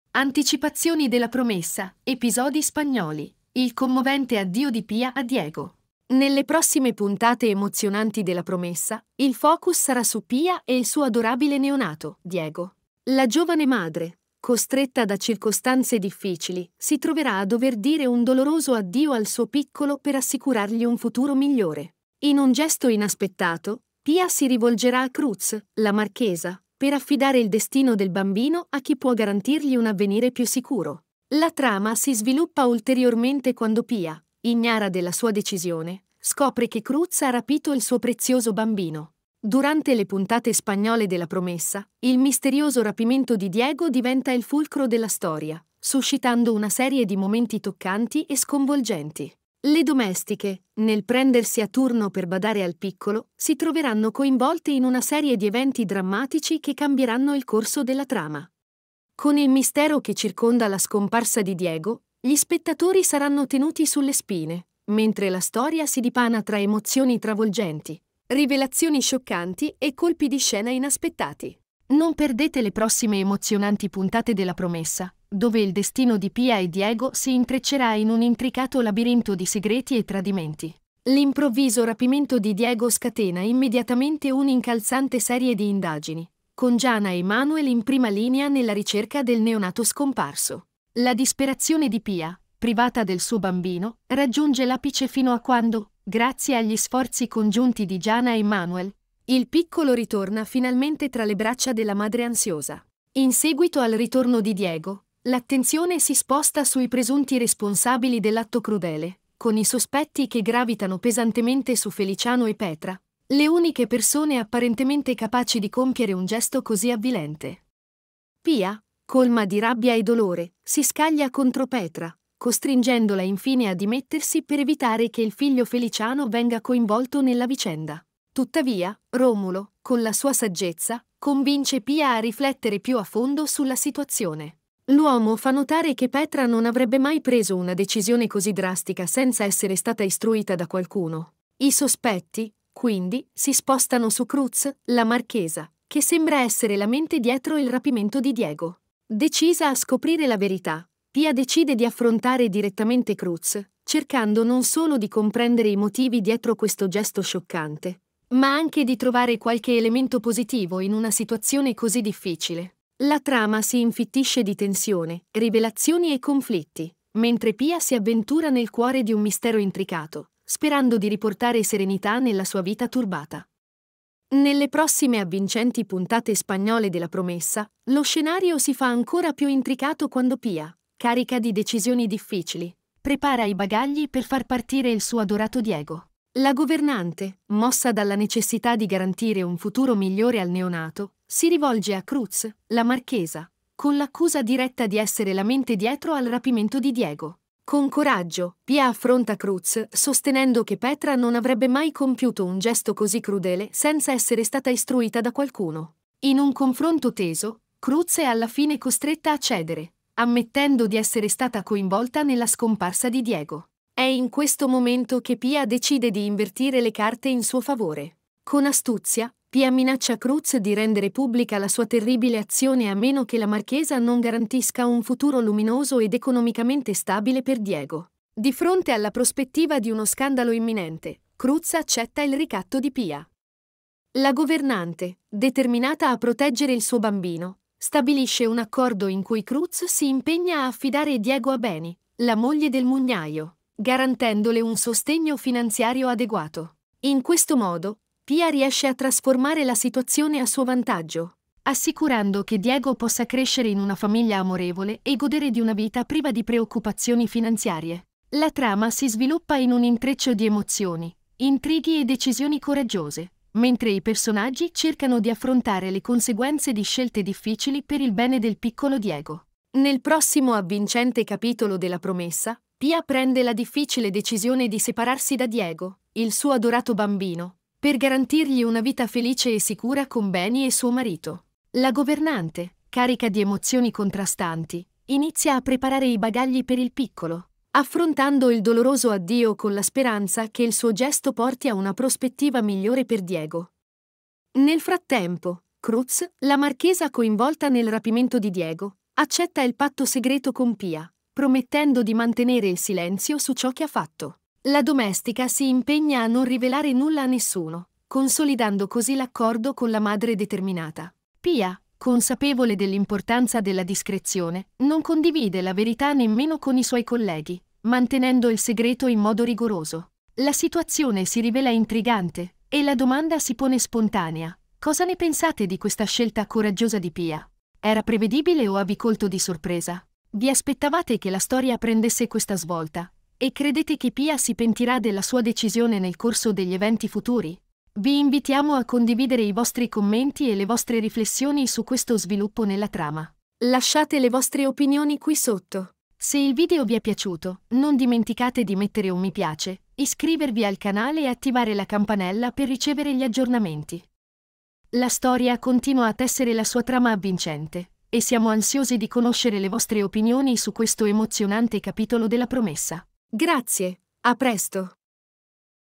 LA PROMESSA Anticipazioni Spagnole | Pia costretta a separarsi da suo figlio! Cruz lo aveva rapito!
Anticipazioni de "La Promessa" - Episodi Spagnoli: Il commovente addio di Pia a Diego.
Nelle prossime puntate emozionanti de "La Promessa", il focus sarà su Pia e il suo adorabile neonato, Diego. La governante, costretta da circostanze difficili, si troverà a dover dire un doloroso addio al suo piccolo per assicurargli un futuro migliore.
In un gesto inaspettato, Pia si rivolgerà a Cruz, la marchesa, per affidare il destino del bambino a chi può garantirgli un avvenire più sicuro.